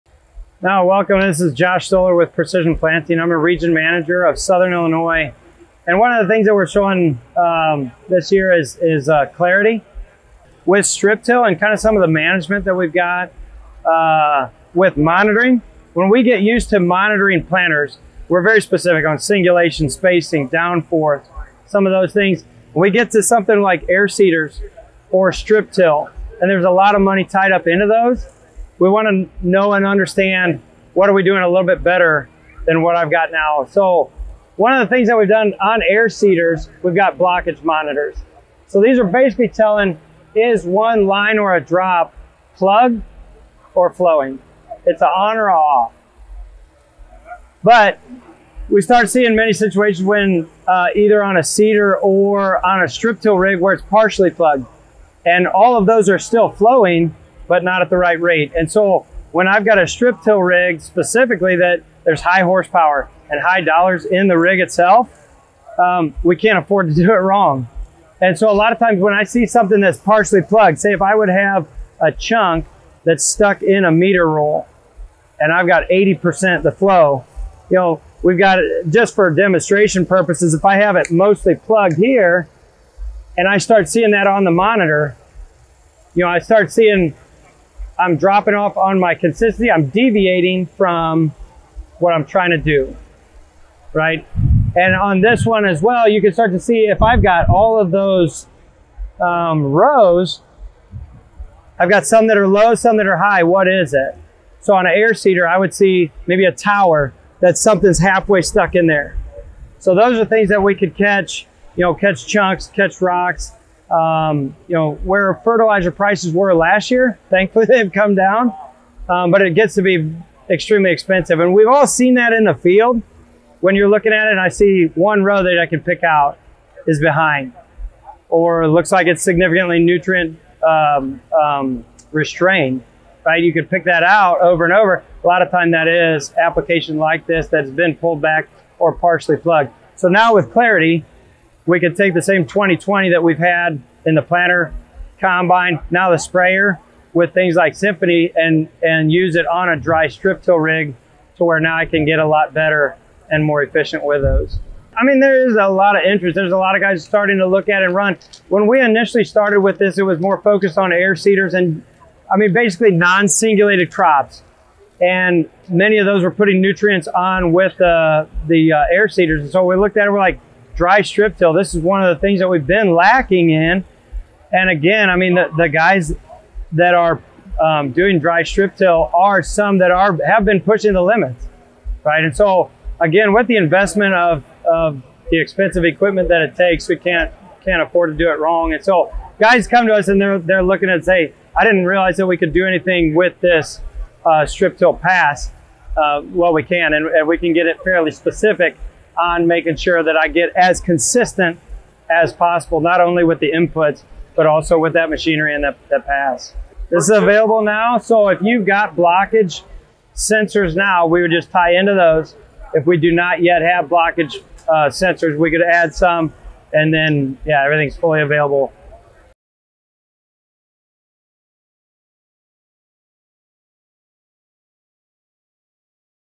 Precision Planting Launches Clarity Blockage & Flow Monitoring System
Precision Planting Regional Manager Josh Stoller displays Clarity, a recently launched blockage and flow monitoring system aimed at helping growers get a better picture of exactly how much seed or dry fertilizer is flowing through the system on a row-by-row level.